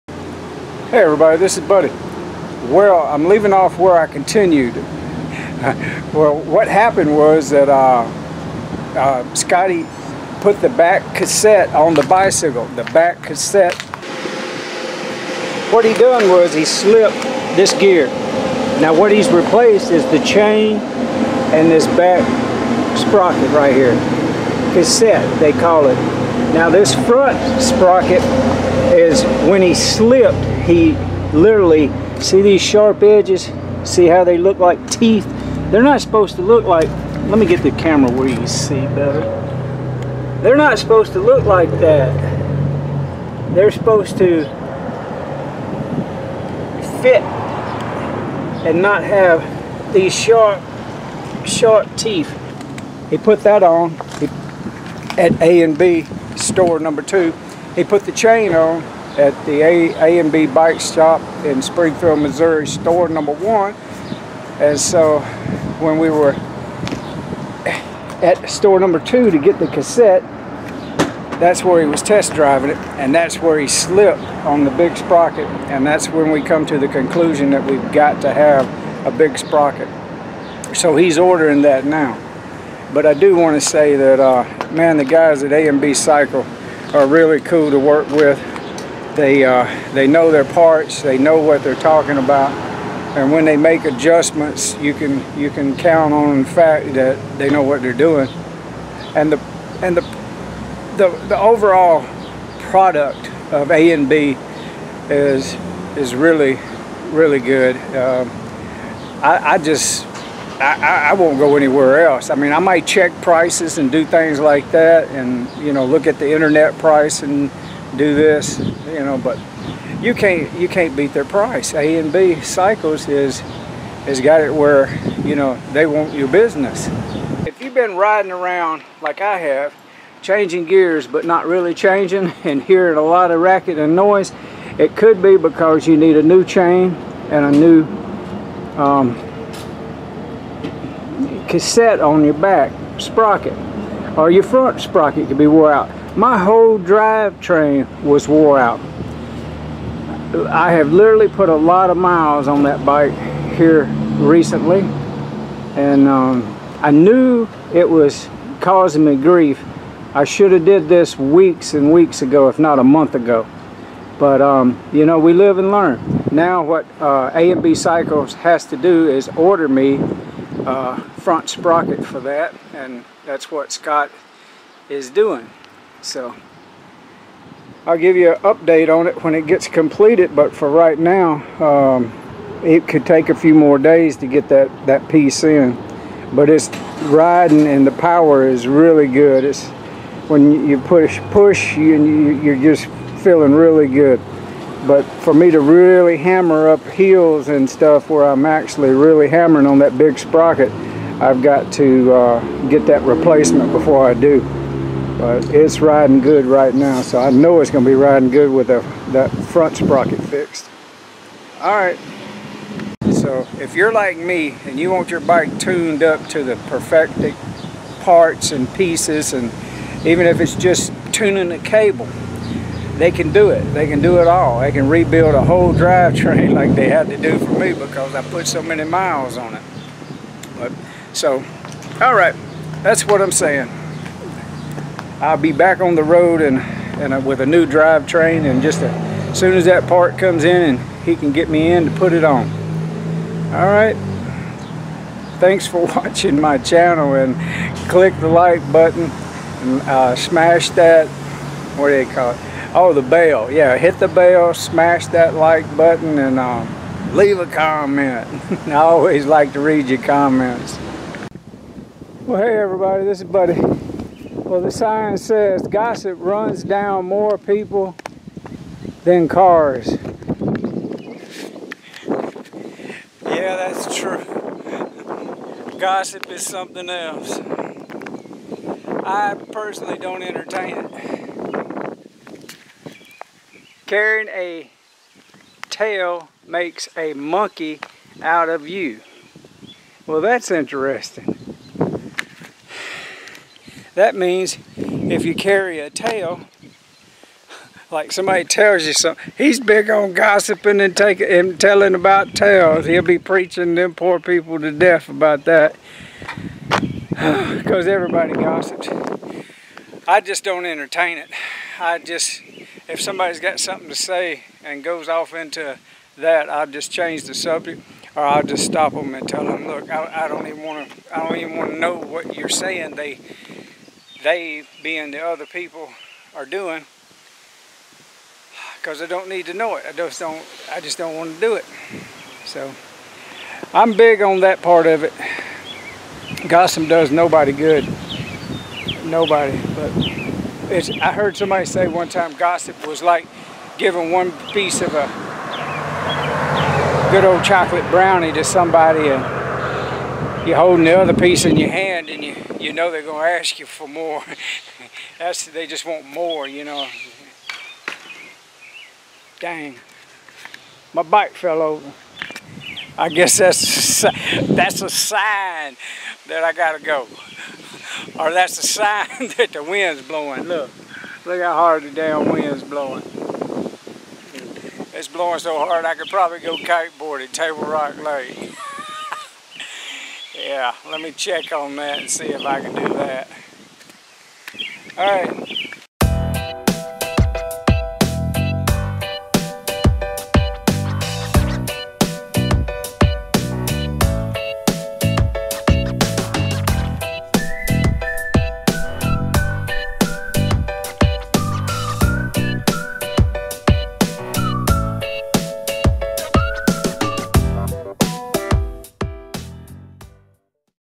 My bicycle drivetrain needs replacing - A&B Cycle Springfield Missouri
The drivetrain on my Road Bike needs replacing.  So I went to A and B Cycle shop in Springfield Missouri.  Based on how they work with me,  in the past they're the best bike shop in Springfield MO in my opinion.

After I get my bike worked on this time I'll do a Review video of how the guys did this time.

A&B Cycle on S National Ave.
3620 South National Avenue
Springfield, MO 65807
(417) 866-BIKE (2453)
Mon - Sat: 10:00am - 6:00pm
Sun: 11:00am - 5:00pm

A&B Cycle on Republic Rd.
2002 E. Republic Road
Springfield, MO 65804
Sun: Closed

~ Buddha   : )